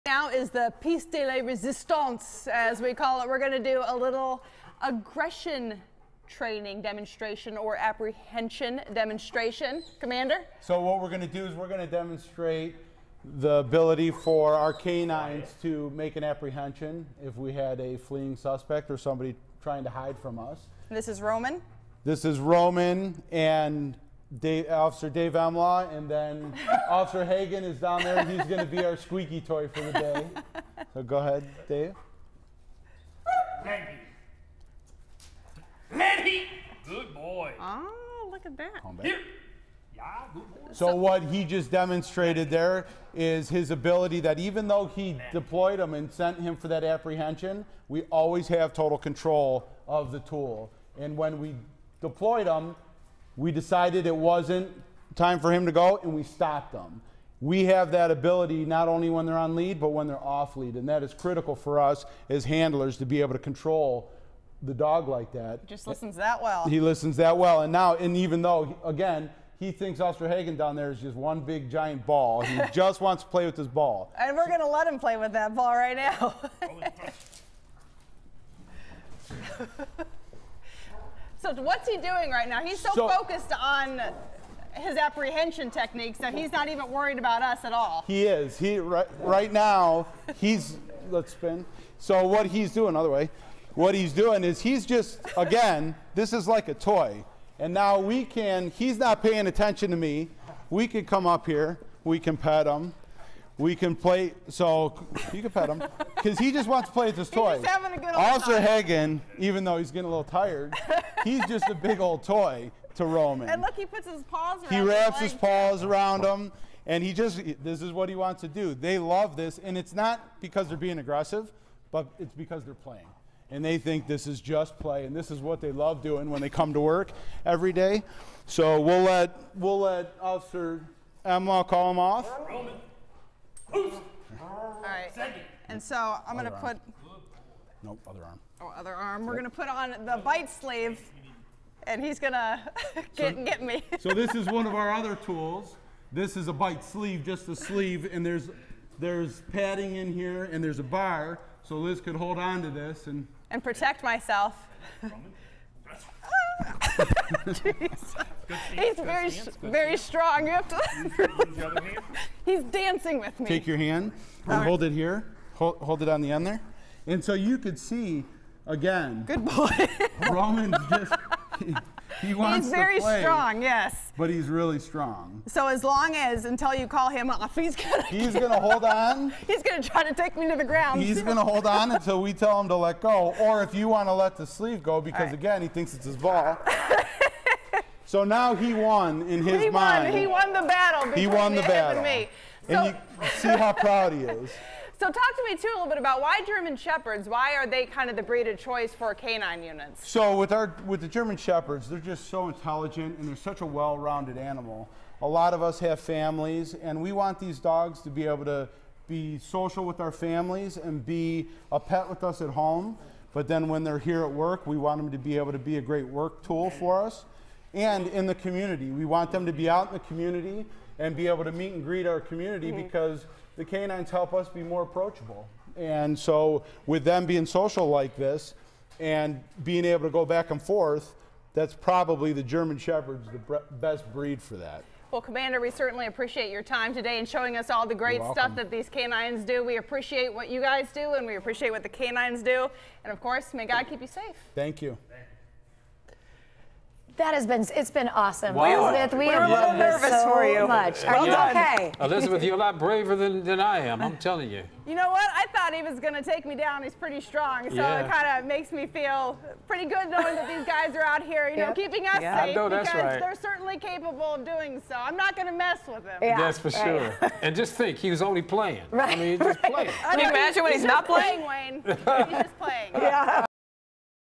Fox 8's Elizabeth Noreika feels just how strong this K-9 unit is
The Shaker Heights Police Department has seen the value of having a specially trained K-9 force for over three decades. The department has one of the largest and oldest K-9 units in suburban Northeastern Ohio. Fox 8's Elizabeth Noreika spent a morning learning more about the role the K-9 unit plays. She also got a chance to feel the strenth of these special safety forces during her visit.